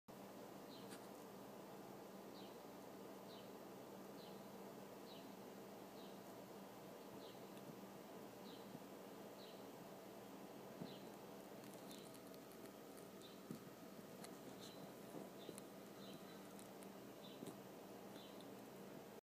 Moths caught mating
I caught these 2 moths having sex and they seen me watching so they flew away. : (